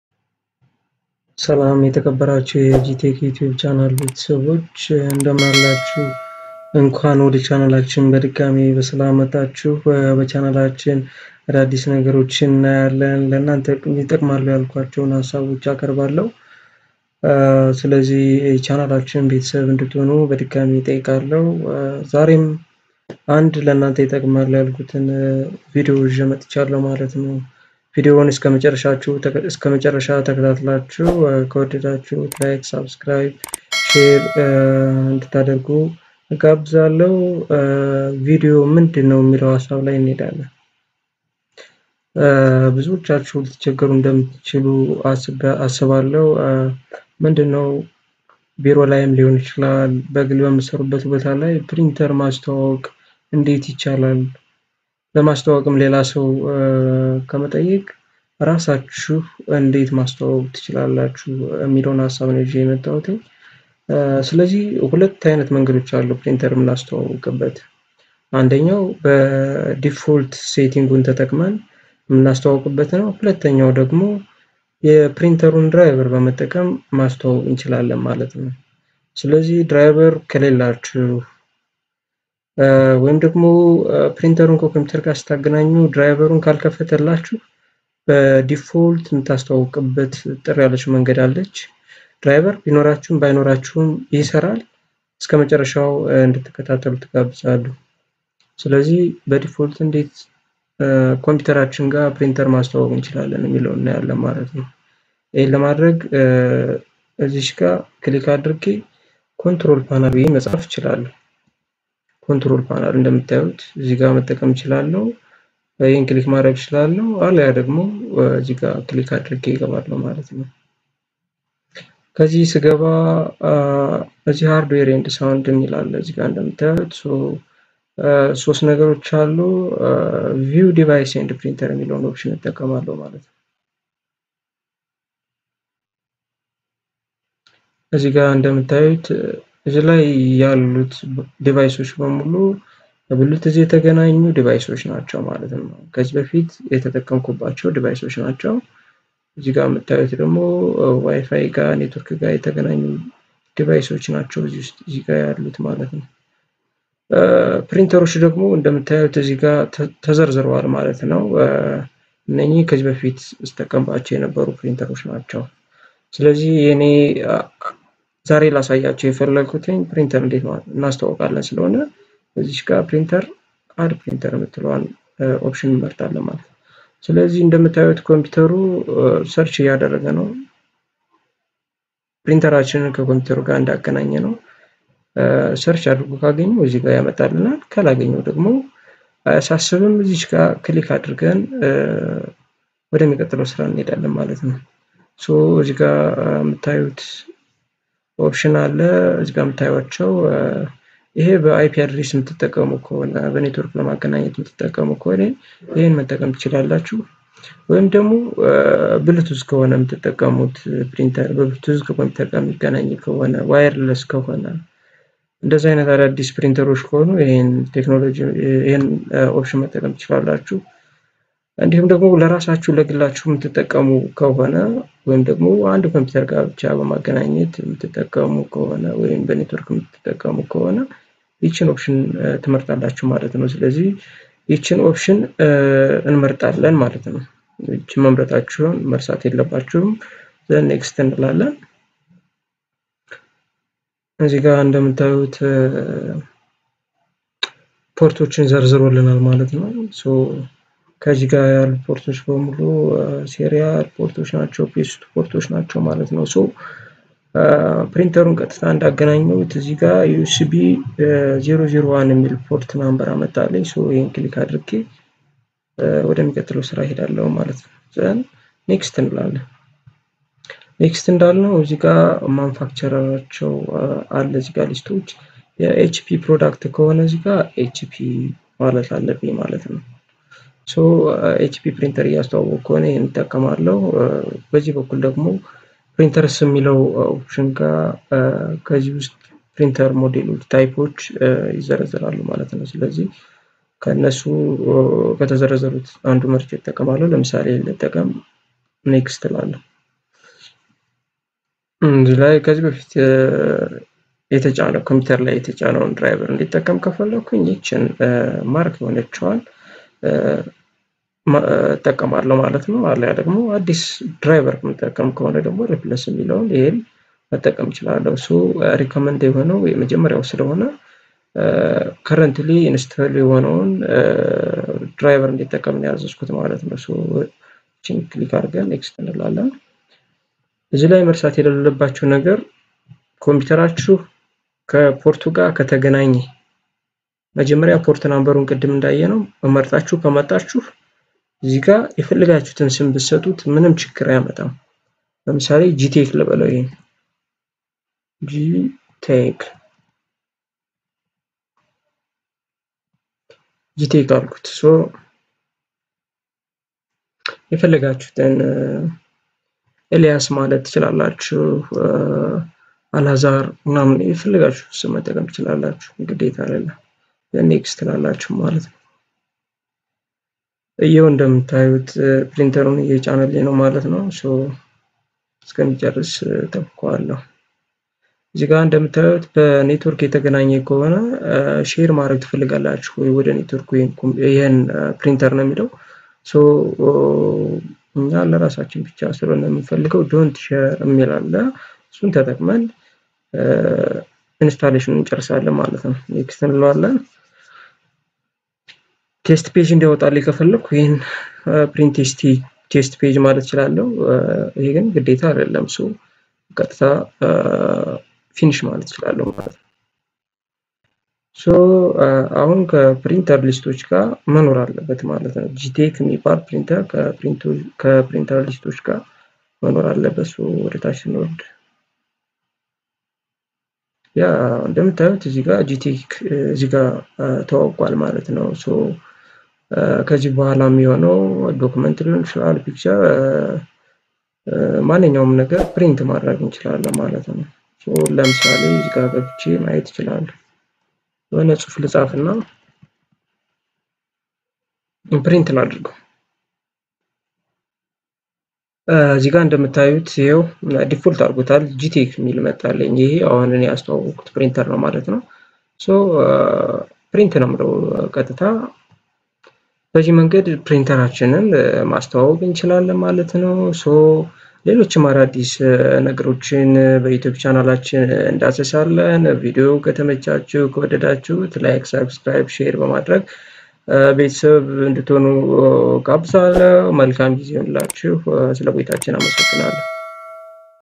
How to configure printer with a computer /እንዴት ፕሪንተር ከኮምፒዩተር ጋር በቀላሉ እናስተዋውቃለን
This video shows easy necessary steps to configure a printer with computers.